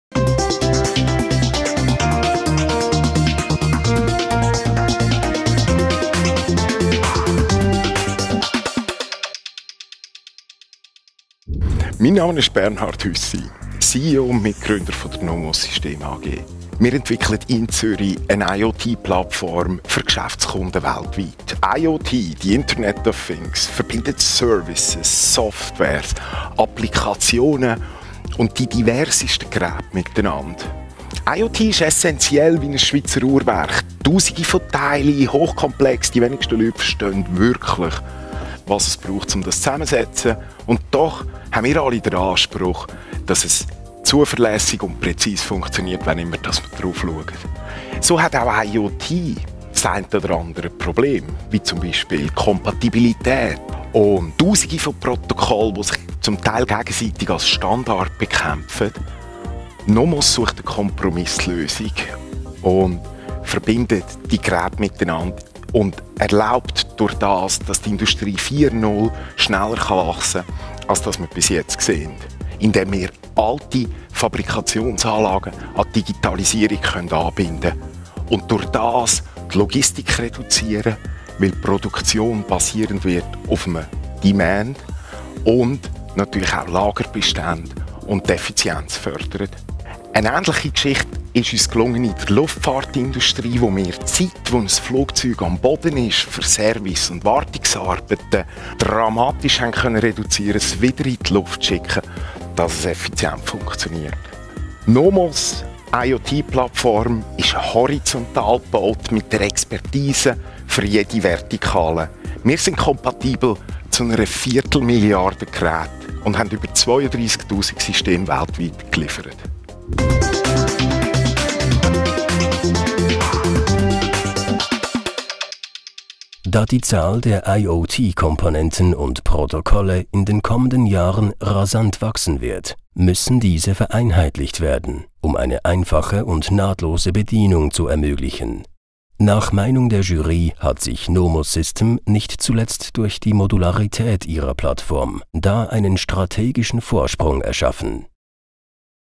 Finalist Swiss ICT Award 2017: Nomos Systems AG mit «Nomos System»
Nomos Systems AG aus Zürich ist mit der universell nutzbaren IoT-Automatisierungs-Software in diesem Jahr bereits mit einem «Red Herring» ausgezeichnet worden.